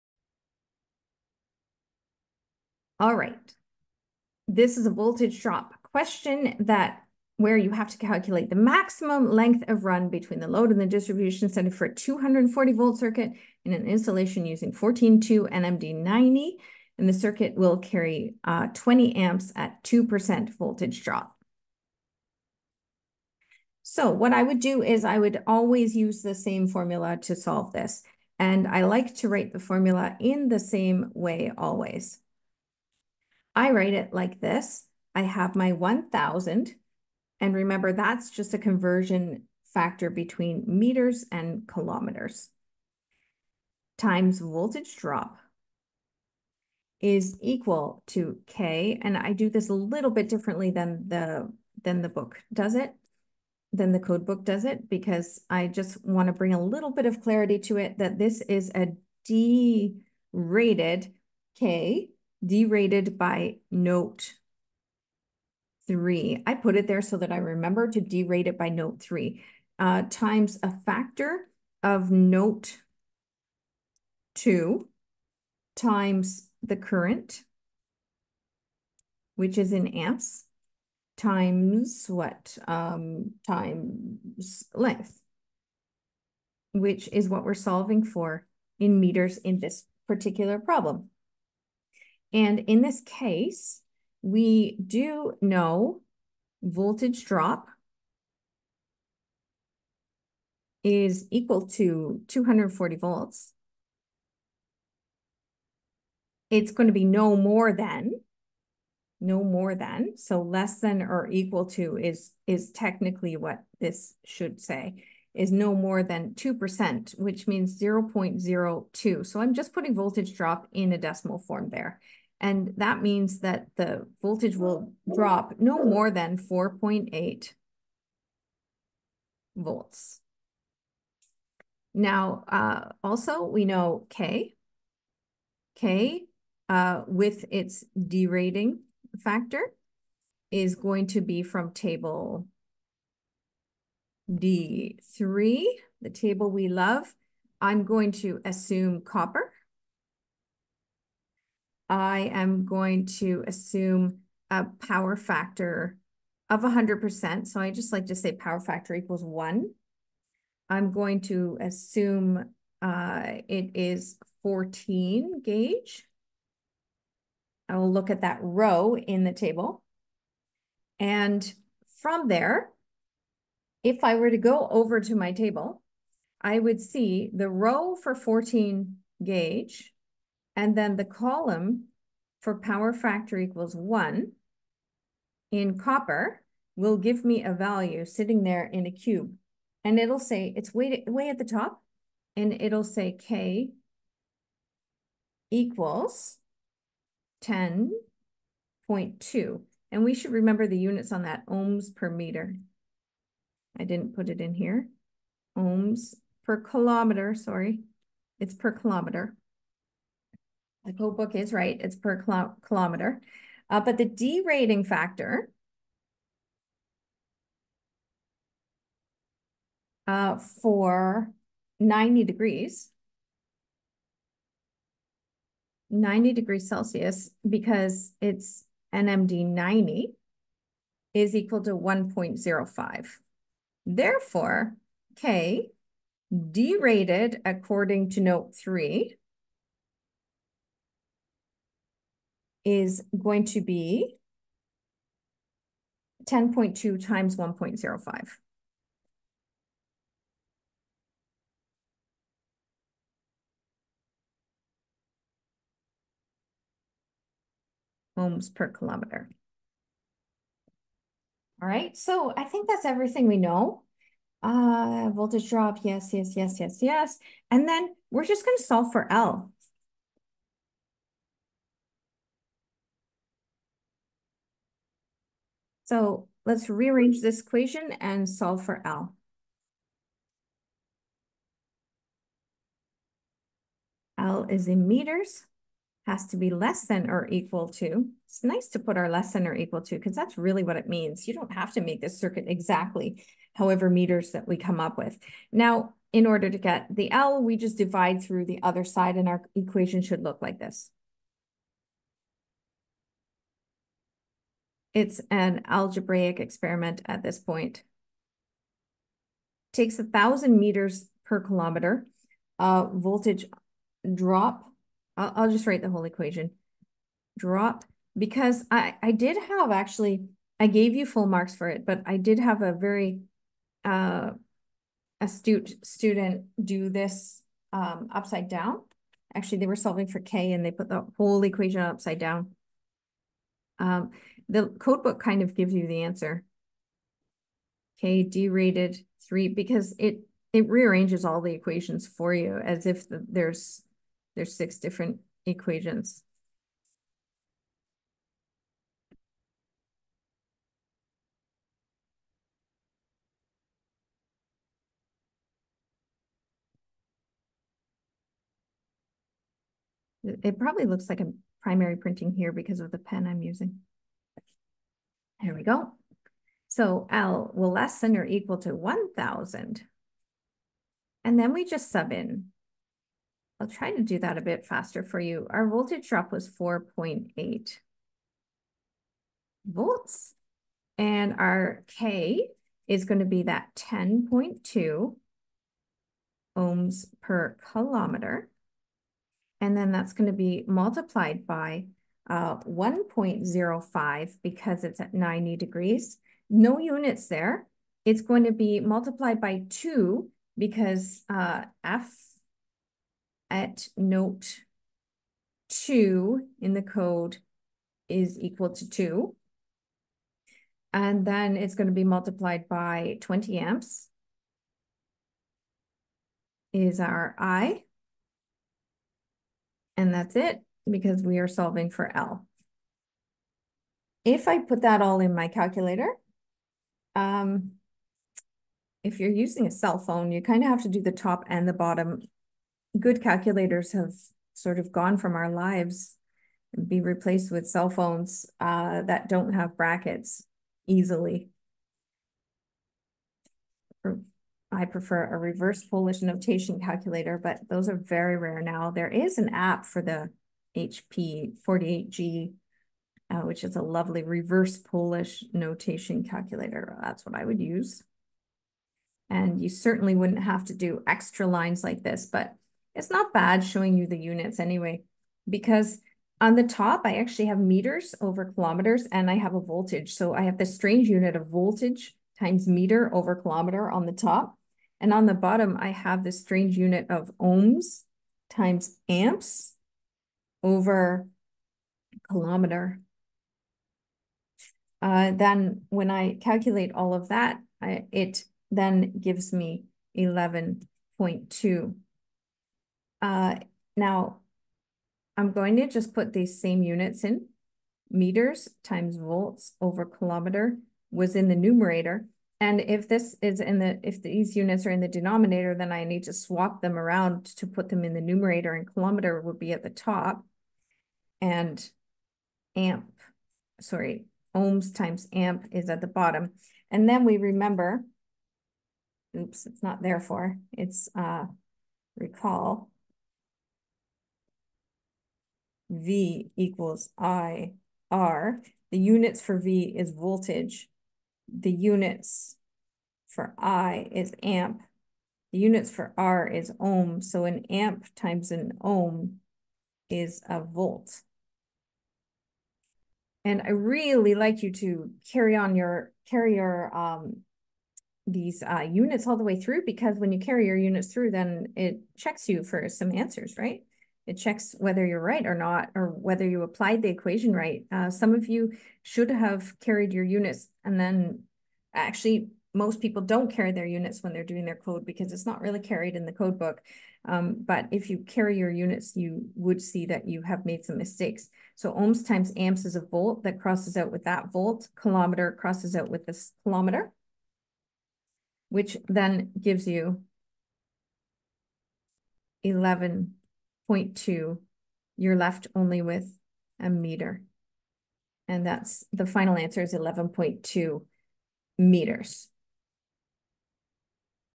Calculate Maximum Length of 14 AWG NMD90 Given Voltage Drop
Example problem with solutions and units for a Voltage Drop question with the new method in the latest Canadian Electrical Code 2024.  The question I answer is as follows:

What is the maximum length of run between the load and the distribution centre for a 240 V circuit in an installation using 14/2 NMD90 as the wiring method?  The circuit is intended to carry 20 A at 2% voltage drop. Assume 100% power factor.

I use Table D3 to solve it.

This is useful for electricalcode